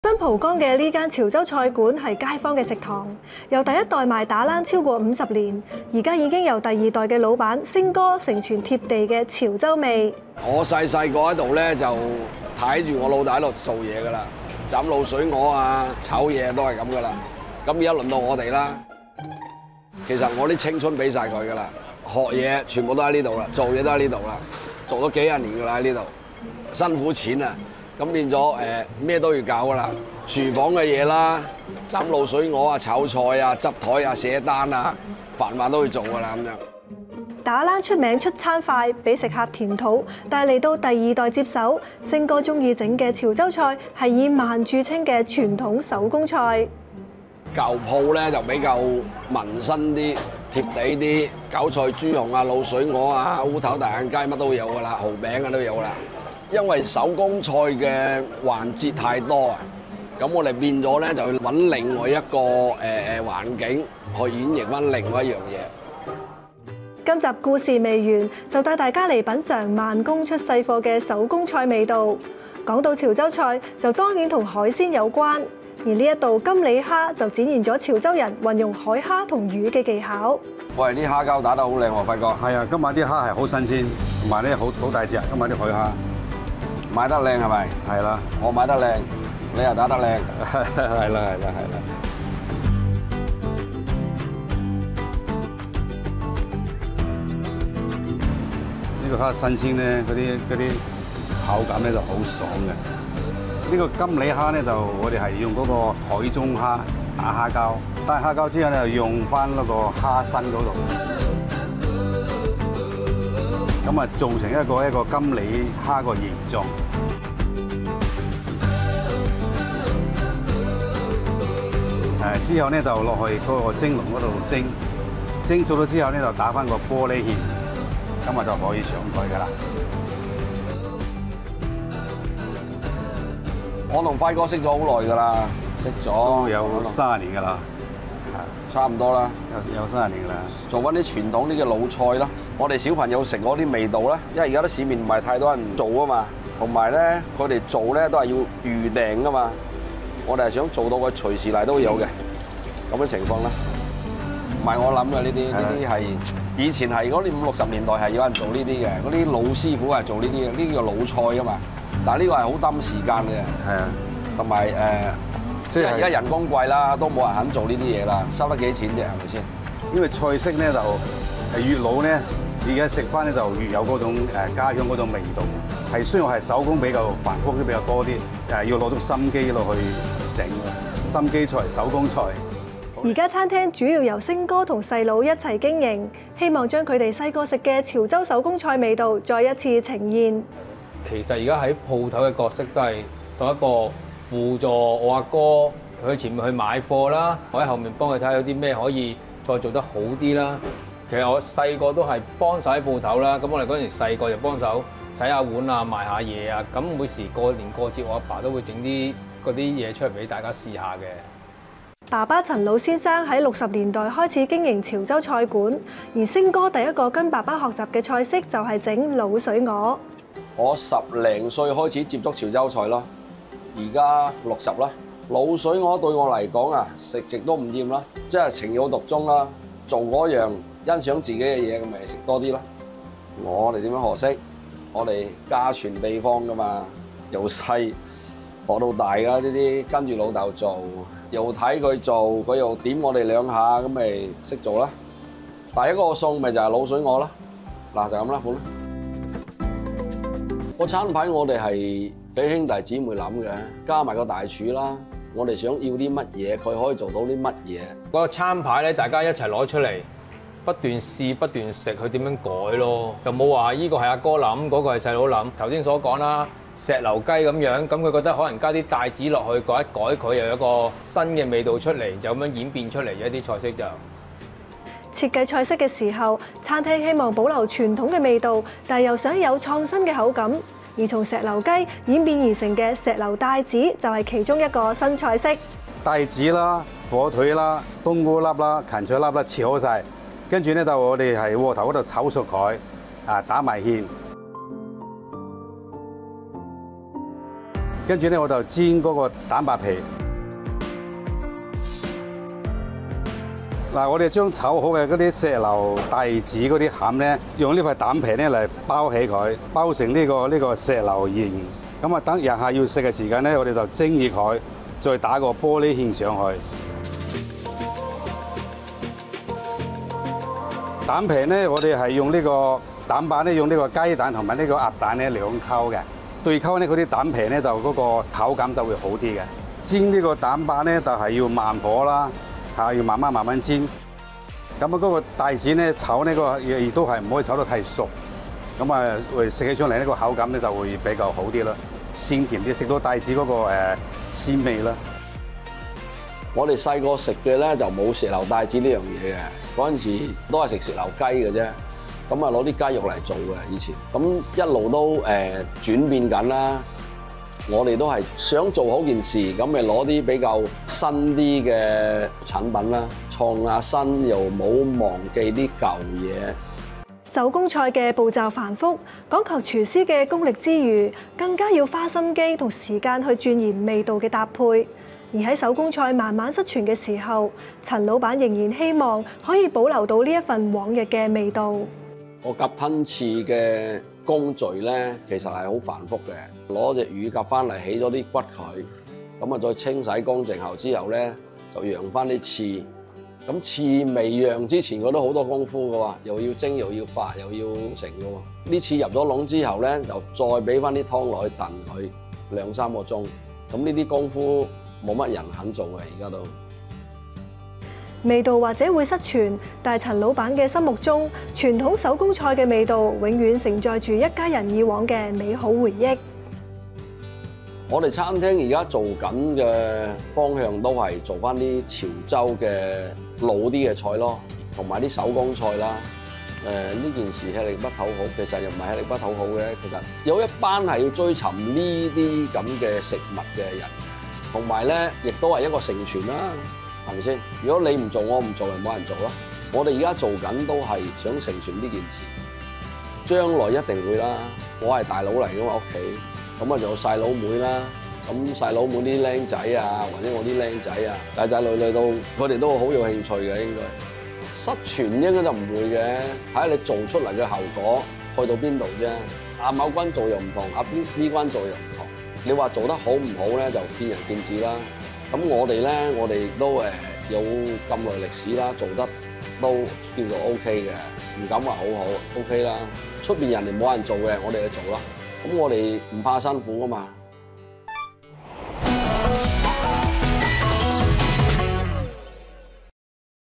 美食新聞報道 ｜太子潮州菜館父傳子承！堅信慢工出細貨：坊間沒有人做的，我們會做，我們不怕辛苦｜TVB Plus
tvbplus 美食新聞報道｜太子潮州菜館父傳子承！堅信慢工出細貨：坊間沒有人做的，我們會做，我們不怕辛苦

新蒲崗一間由60年代開始經營的潮州菜館，主打民生菜式。到第二代老闆陳星接手時，決定為菜館開創一條新路，於太子開設分店，專攻工序繁複的傳統手工菜。「這是小時候的味道，現時已很少人會做這些菜了，就算有都多數要預訂。我們希望可以做到，客人隨時來都可以食到。」

跟陳星一同經營的，還有弟弟阿輝，他表示：「我的角色都是輔助哥哥，幫手看看可以怎樣做得更好。」店內的餐牌是幾兄弟姊妹跟大廚一同構想，每道菜都充滿心思，陳星補充：「有很多傳統潮州菜，年輕人未必試過。如果可以在我們這裡試過覺得好味的話，就會有『回頭』的效果。」

陳儀興尚潮樓
太子太子道西179號地舖及1樓